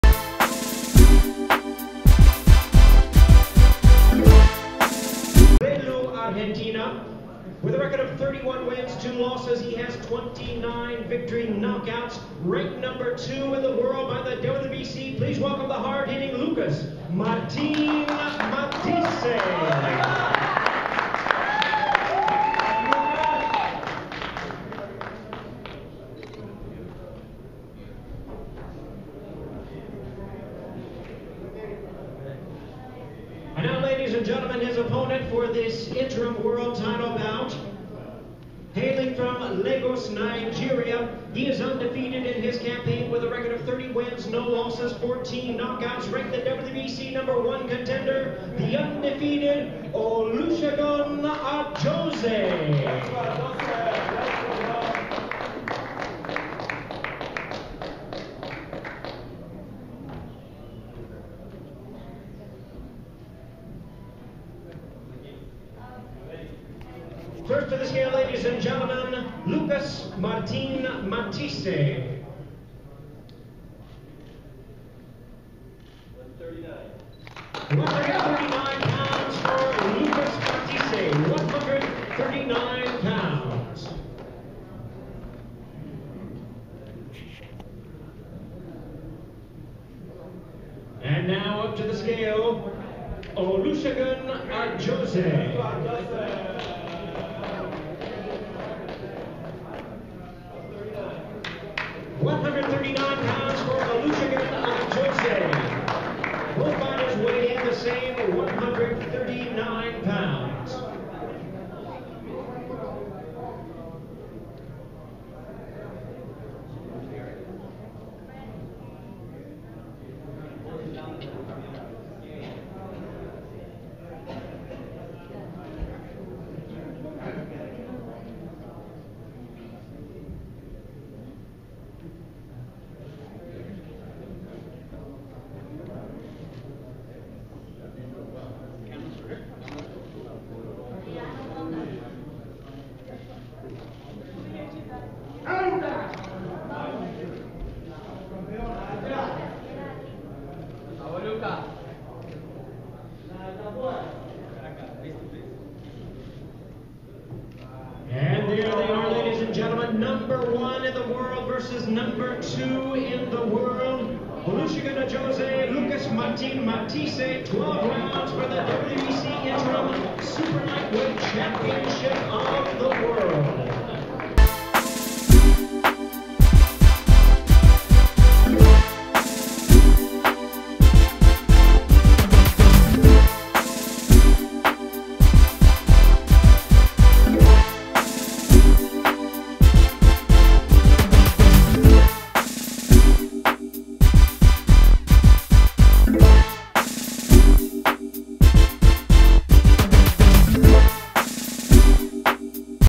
Weigh-in coverage: Lucas Matthysse (139 lbs.) vs. Olusegun Ajose (139 lbs.)
Chris Robinson was on hand at the Hard Rock Hotel and Casino in Las Vegas, Nevada as Lucas Matthysse and Olusegun Ajose weighed in ahead of their clash on Saturday, Sept. 8...